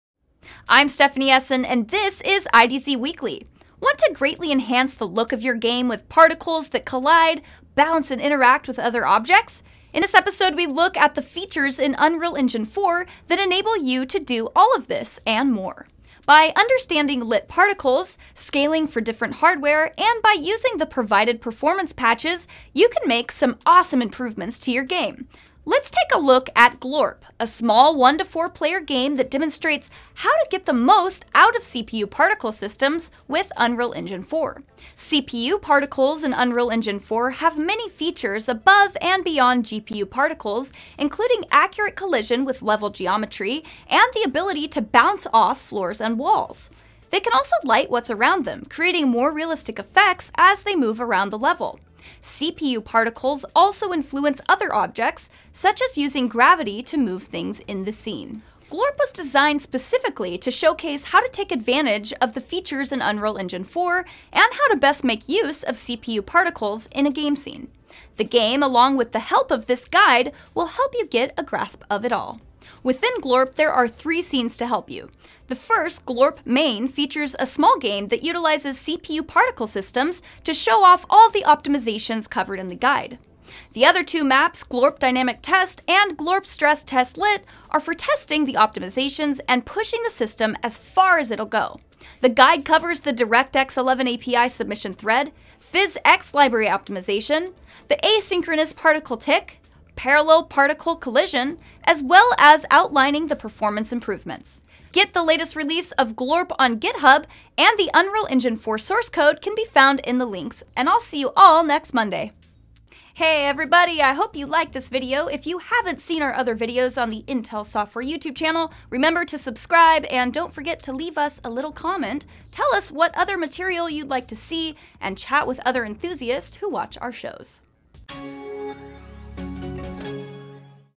Maximizing Visuals with CPU Particles in Unreal Engine* 4 | IDZ Weekly | Intel Software
Do You want to greatly enhance the look of your game with particles that collide, bounce, and interact with other objects? In this episode, You'll see some of the features in Unreal Engine 4* that enable you to do all of this, and more! Let's GLORP!!!

Read the guide “Maximizing Visuals with CPU Particles in Unreal Engine* 4”